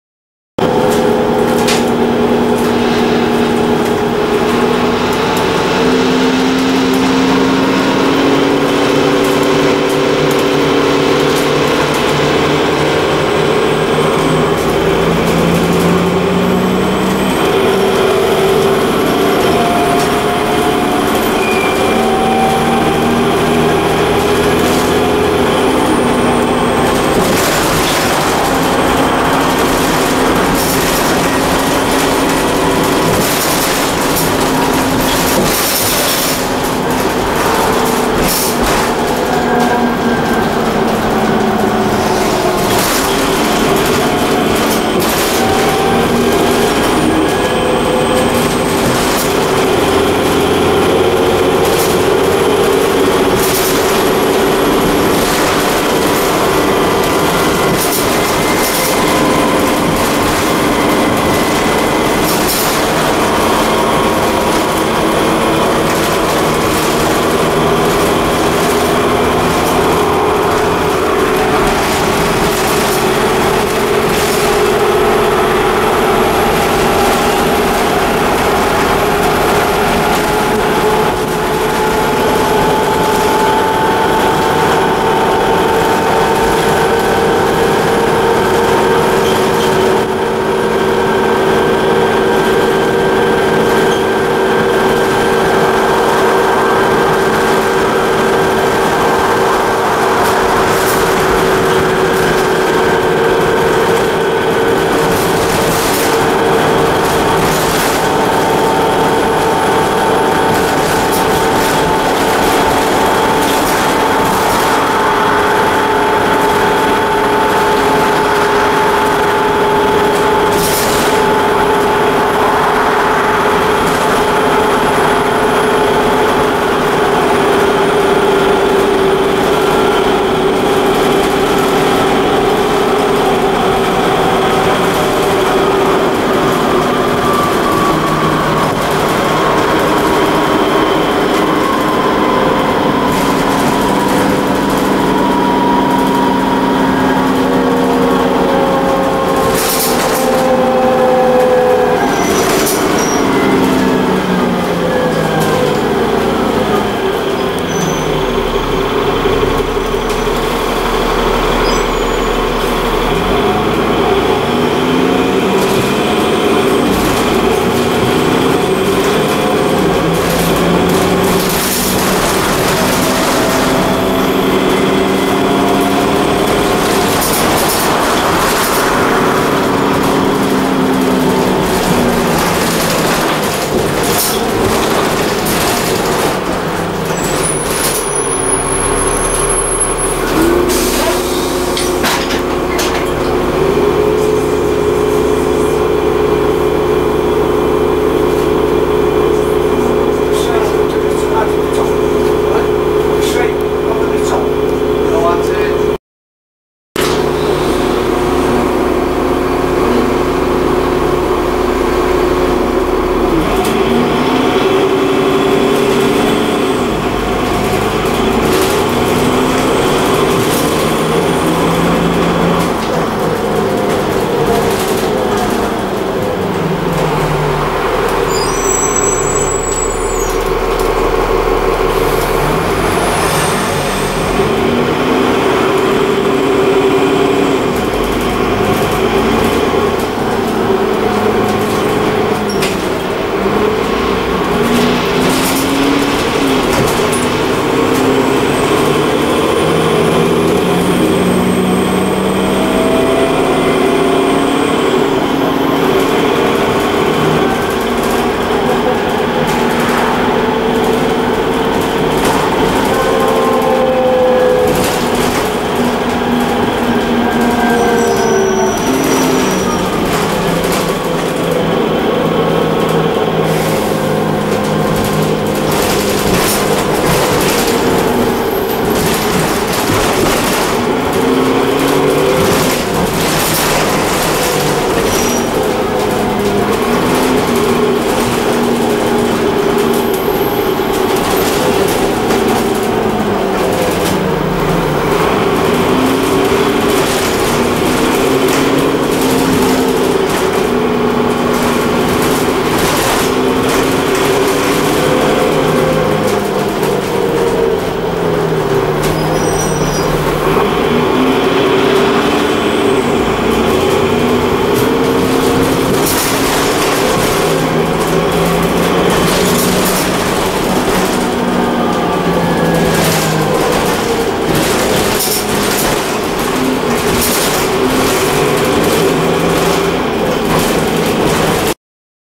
First South Yorkshire 33127 LT02 NVP
Service 70 from Catcliffe Filmed 26/07/14
sorry about the effects stupid video software